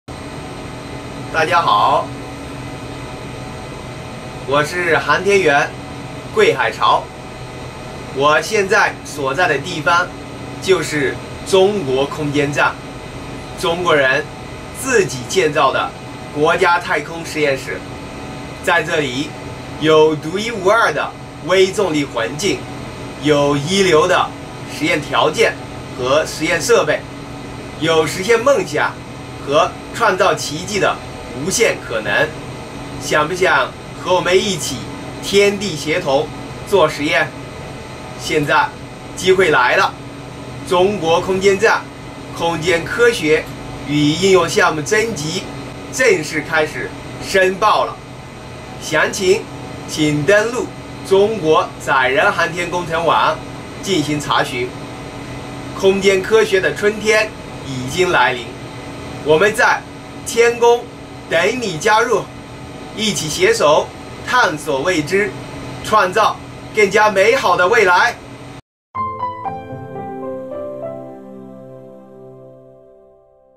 航天员桂海潮邀科技界同仁一起到“天宫”做实验
中国空间站空间科学与应用项目申报系统于北京时间2023年7月20日上午正式开通，正在空间站执行任务的我国首位载荷专家桂海潮发来邀请，诚邀科技界同仁一起到“天宫”做实验。（马帅莎 视频来源 中国载人航天工程办公室）